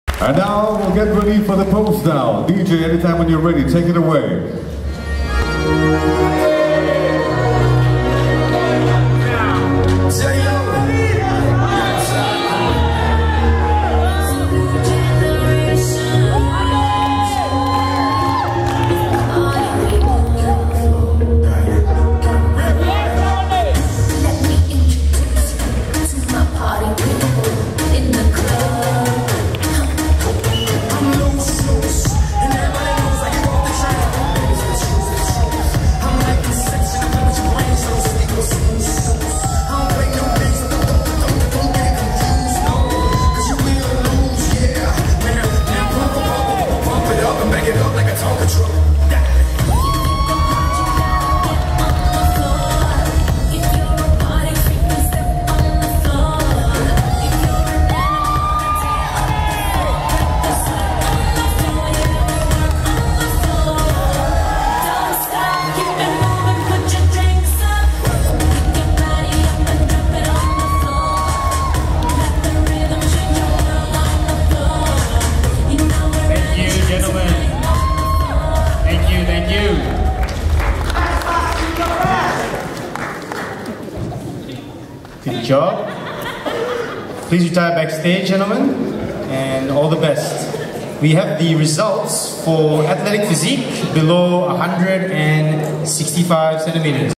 SBPF Nationals 2013 - Pose Down (Below 75kg)
SBPF Nationals 2013
Below 75kg - Pose Down

***Competitors:
#6  Rajana Kumar (4th)
#9  Mohd Iswandy (2nd)
#36 Max Lin (Champion)
#40 Micah Lim (3rd)

13 April 2013 (Saturday) 2pm
University Cultural Centre Hall